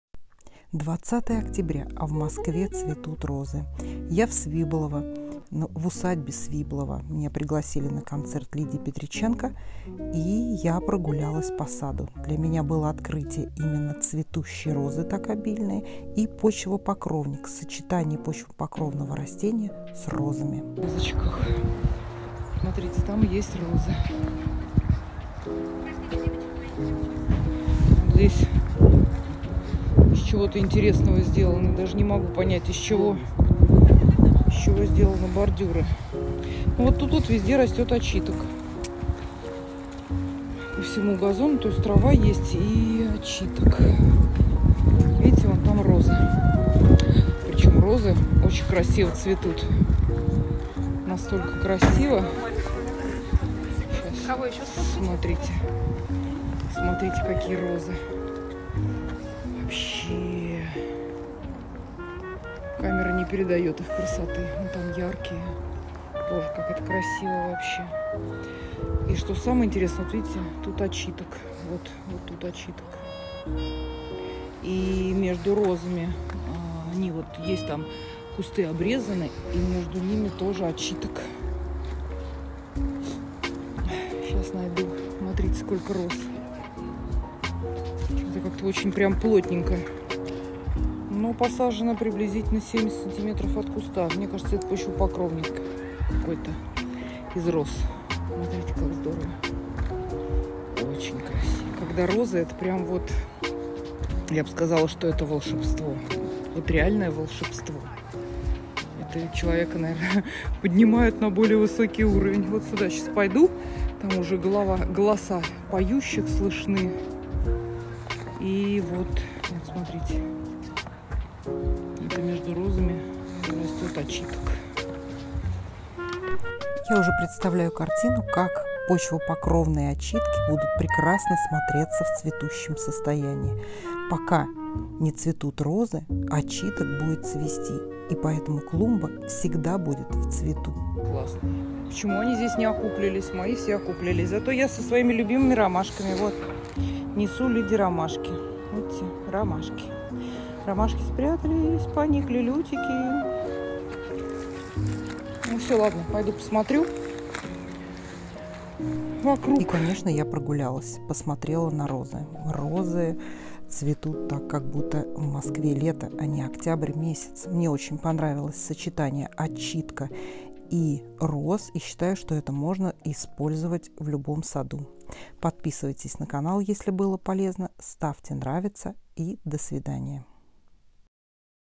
Розы без сорняков / Неожиданное сочетание
В конце октября была в усадьбе  Свиблово в Москве. Поразило пышное цветение роз.